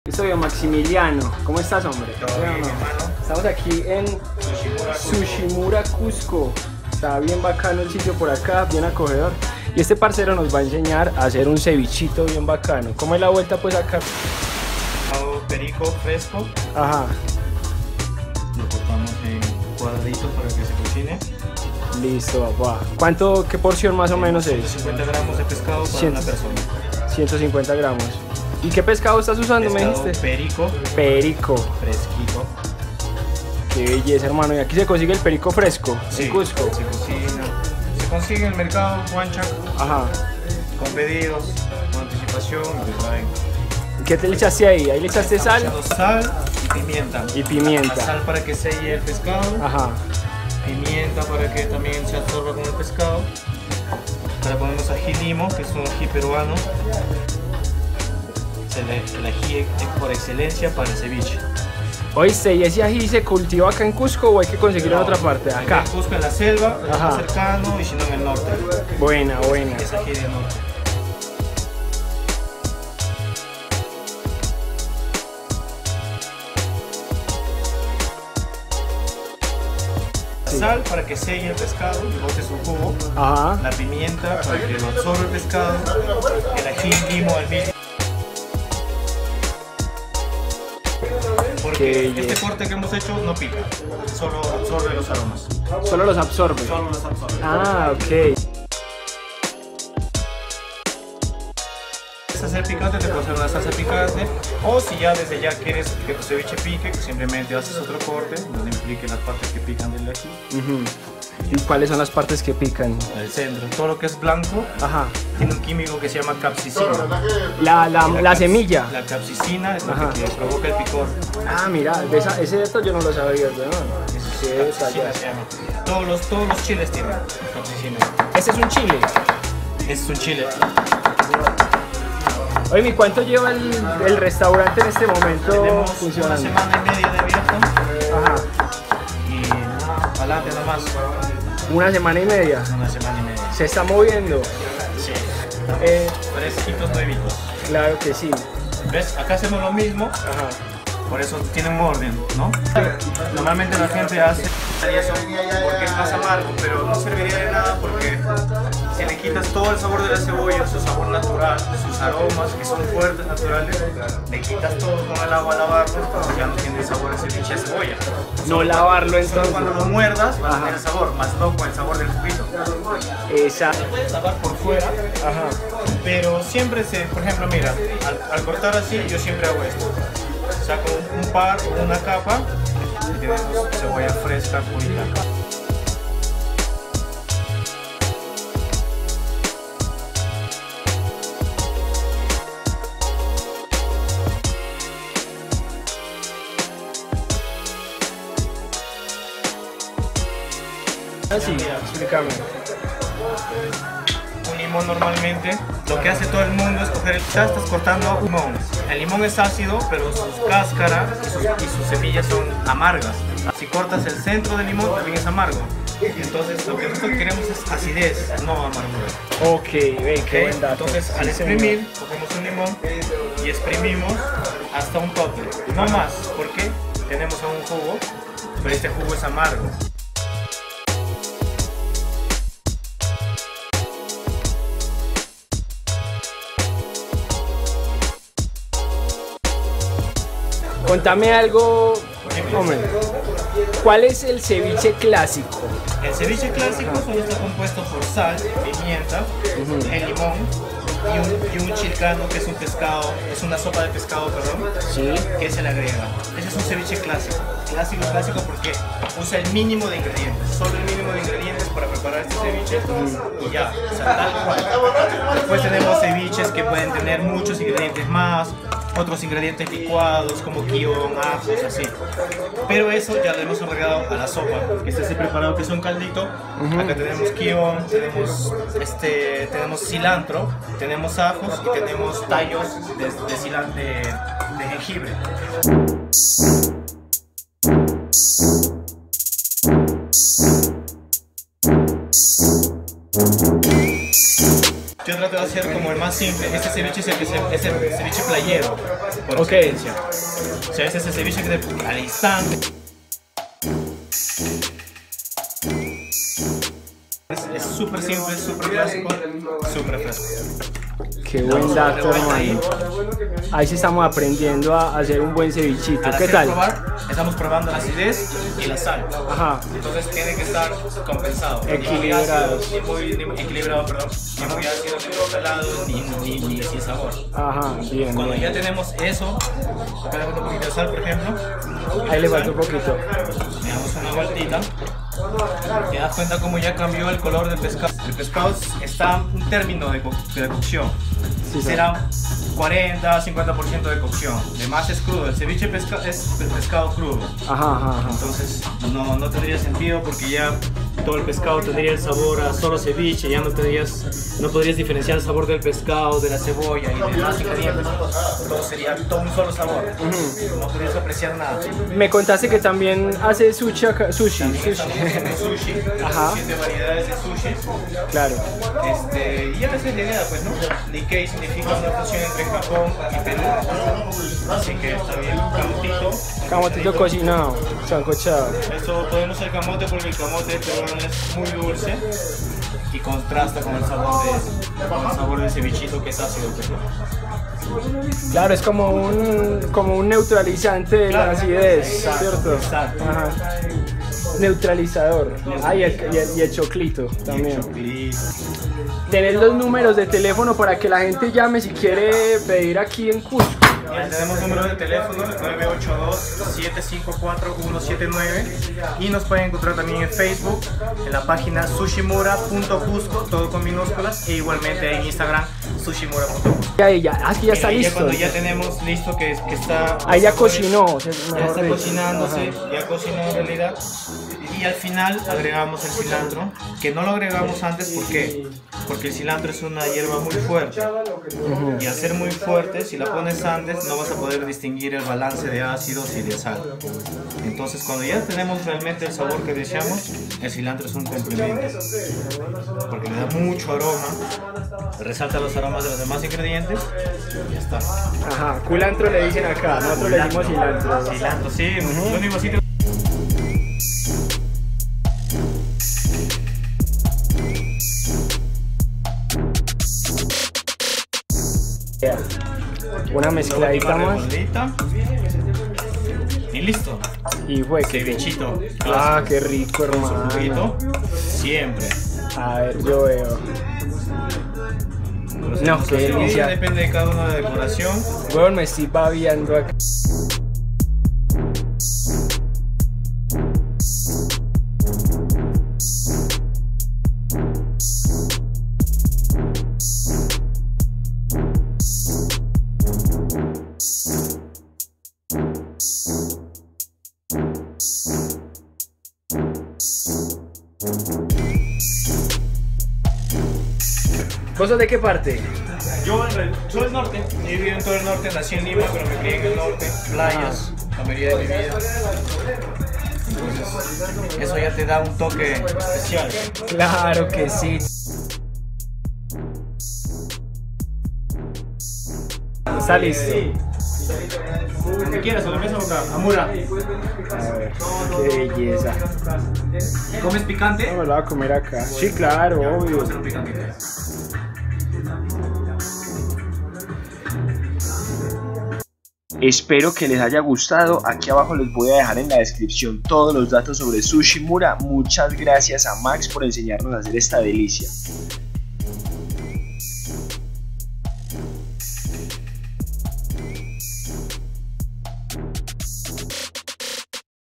CEVICHE PERUANO PASO A PASO
Cómo Preparar el mejor Ceviche de Pescado?? Peruano
 es fácil tal y como nos lo explica Max del Restaurante Sushi-Mura Cusco.
Me desglosó cada uno de lo que necesitamos para hacer un delicioso ceviche clásico peruano.